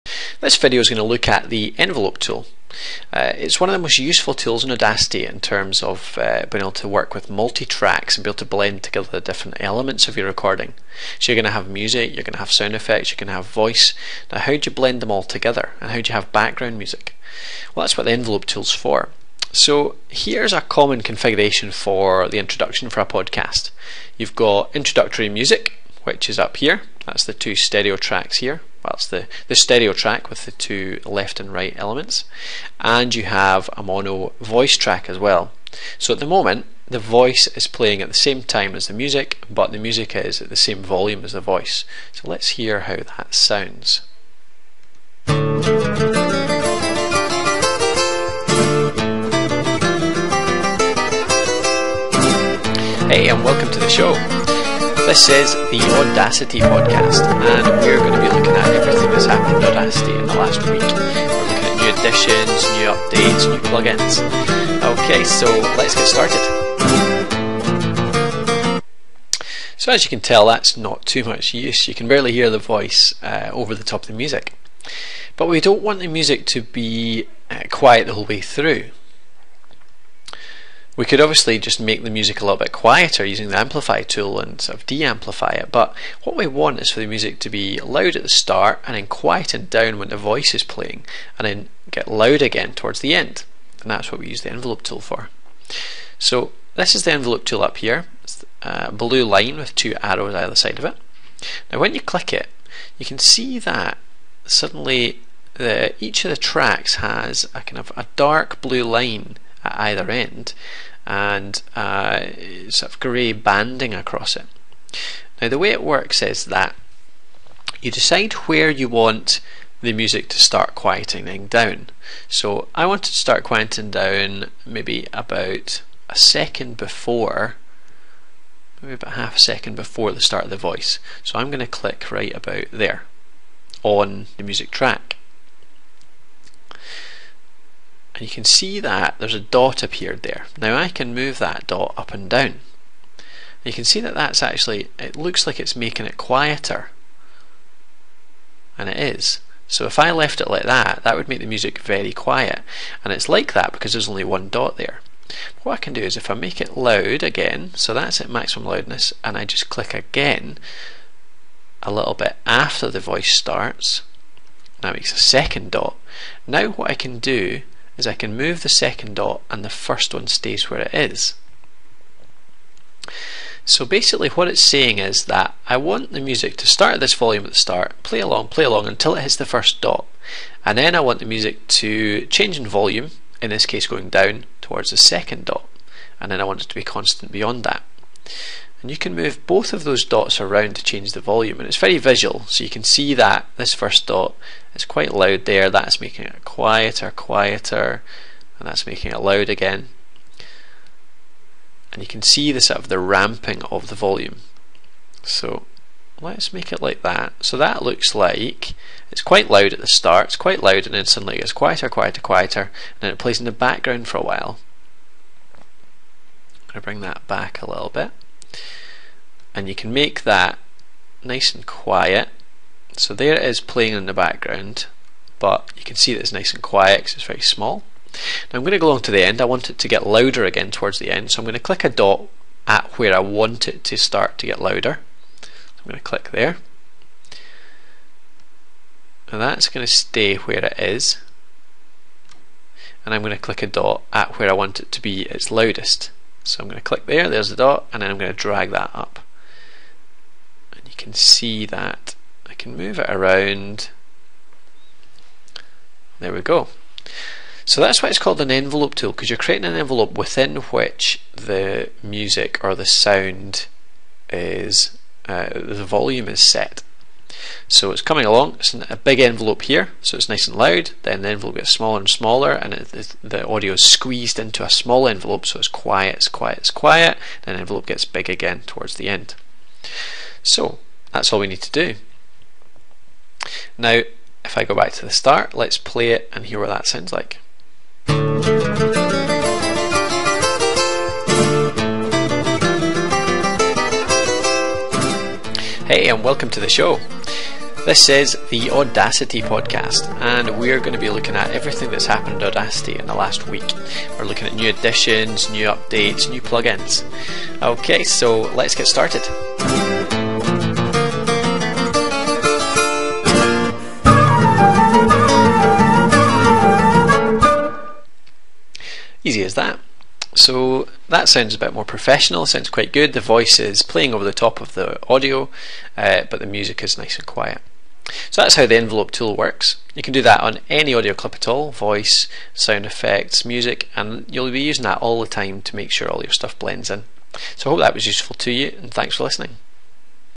Audacity Envelope Tool: Increase or Reduce Your Audio Volume
This video takes you through how the audacity envelope tool works, and how it can be used to play around with the volume of your audio recordings. The envelope tool is idea for mixing sounds together, reducing background music volume and ensuring voice can be heard over the top.

You can find more tutorial videos, or advice on audio editing and recording